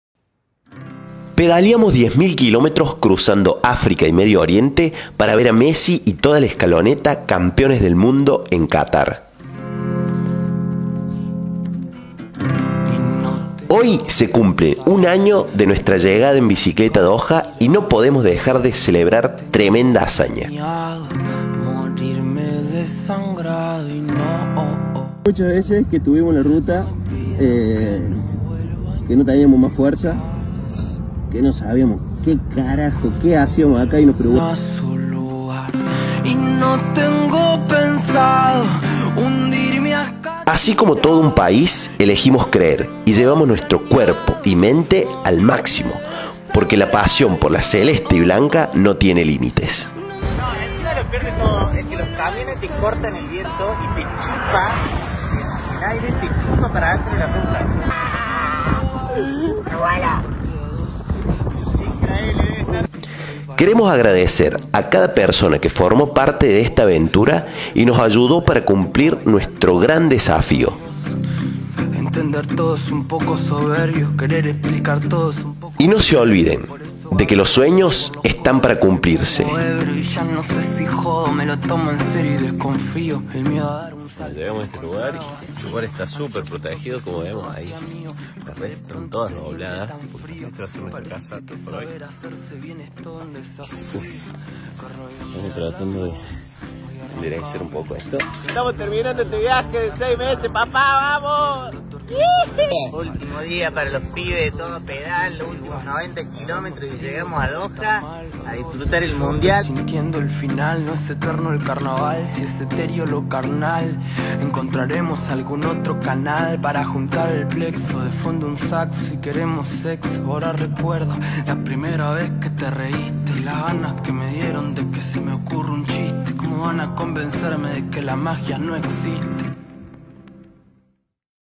VIDEO DE
Se cumple un año de aquel día que entramos a Doha, Qatar🇶🇦 en bicicleta.
🇦🇷Después de haber pedaleado 10.000 km por África y Media Oriente, con la misma ilusión de todo un país.
🌍Fueron 6 meses que nos marcaron para el resto de nuestras vidas. De ahí en adelante todo lo que ocurrió fue magia pura.